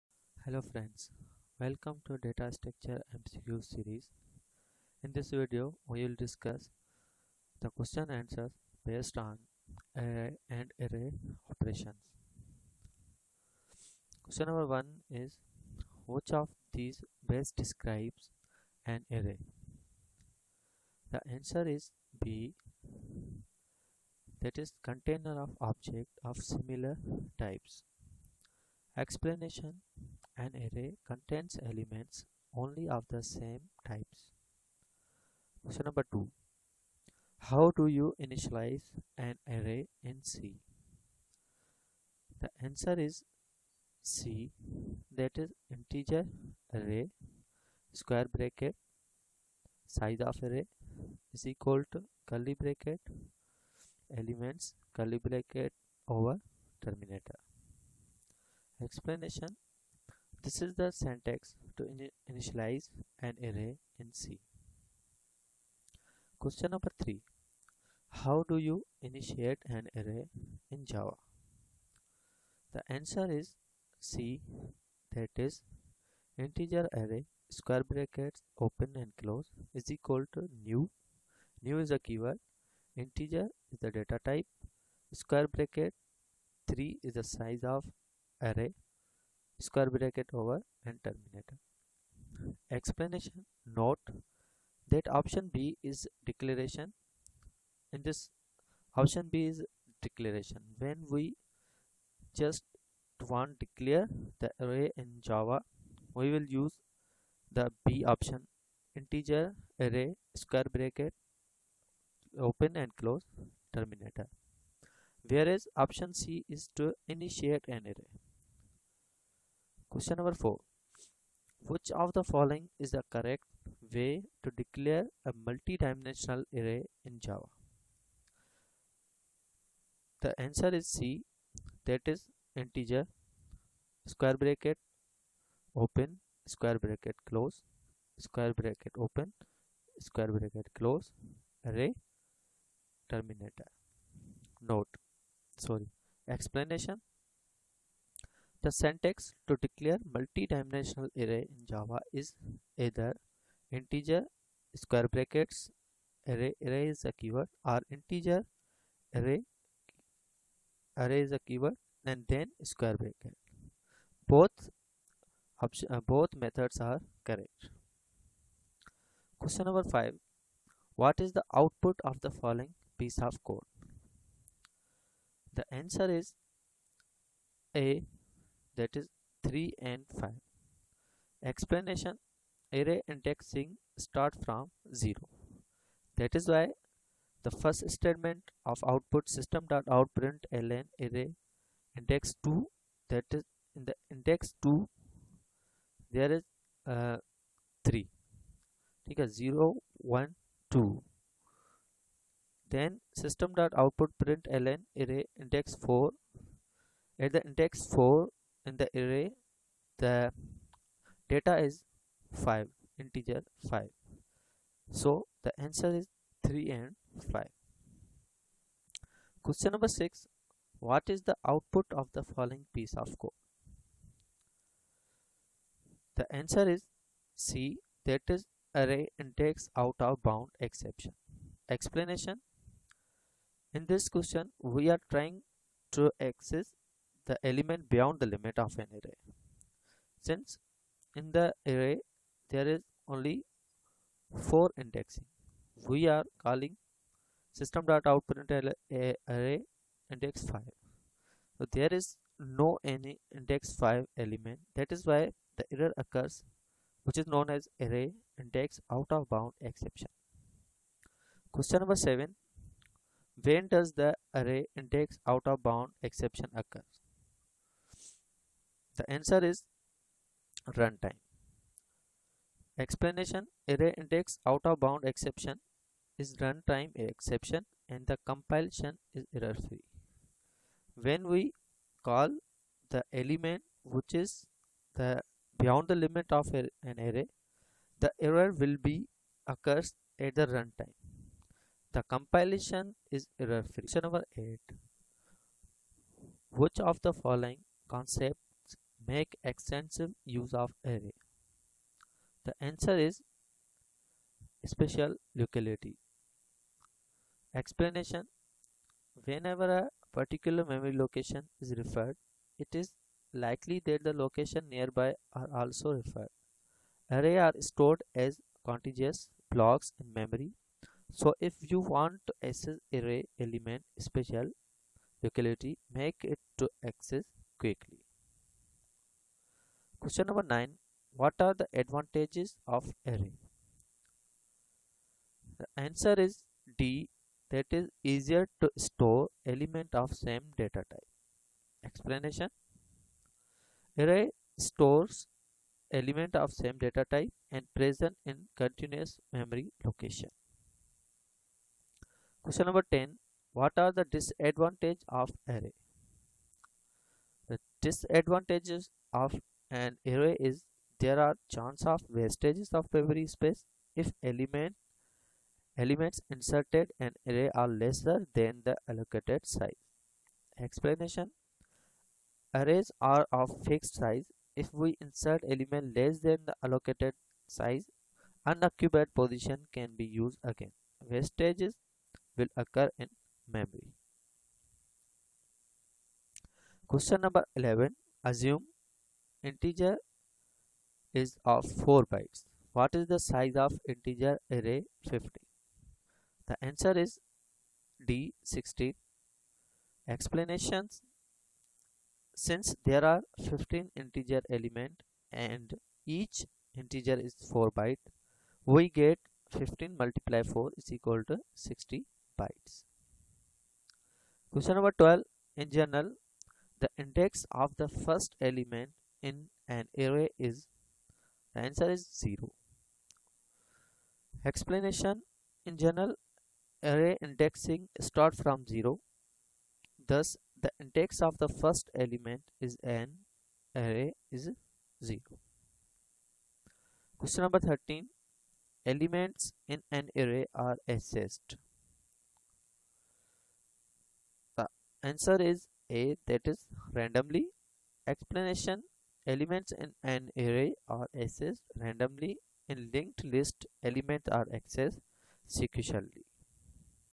spsc computer science lecturer - Data Structure MCQS with Explanations
This set of Data Structure Multiple Choice Questions & Answers (MCQs) focuses on “Array and Array Operations”
Most important and reparative Question and Answers of Data Structure for all type of Computer Science Test .

Most Repeated Computer Science -  Data Structure MCQs with Explanations  SET 1
Our 1000+ “Data Structure – Part 1” (along with 1000+ “Data Structures & Algorithms – Part 2”) questions and answers focuses on all areas of Data Structure covering 200+ topics in Data Structure.